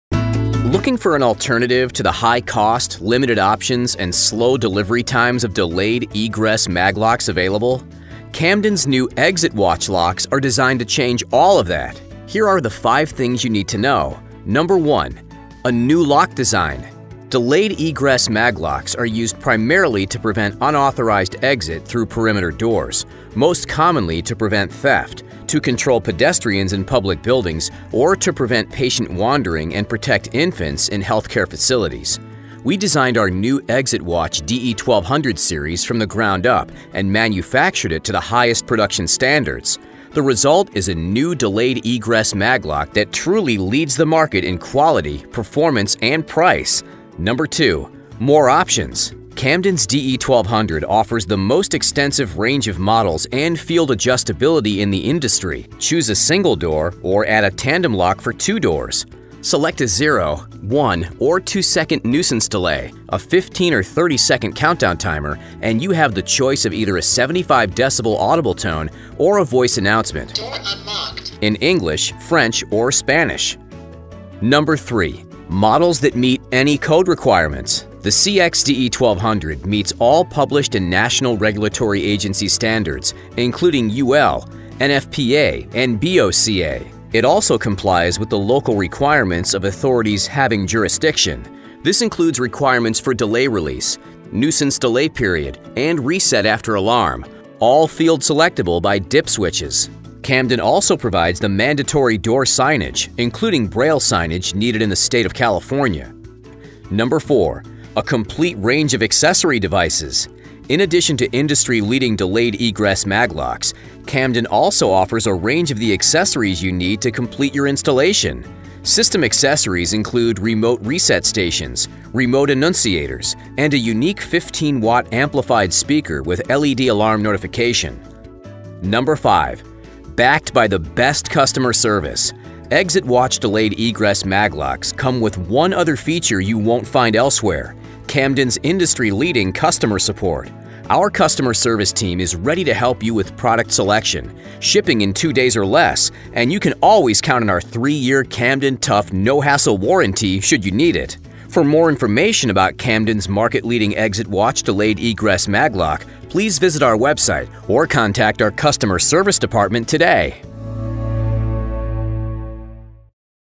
CX-DE1200 Delayed Egress Magnetic Locks - 5 Things You Need to Know!
Watch to learn the top 5 reasons why Camden's CX-DE1200 is the best Delayed Egress MagLock for your installation.

CX-DE1200 Series Single and
 Double Door delayed egress
 magnetic locks are designed
 to provide the best performance
 and value in healthcare,
 childcare, retail, and
 institutional applications.

The innovative human voice and LED
countdown timer offers immediate
 comprehension of status for building
 occupants. These rugged locks are
 available in a range of models, with
factory default operation, to meet NFPA,
 BOCA and local AHJ requirements.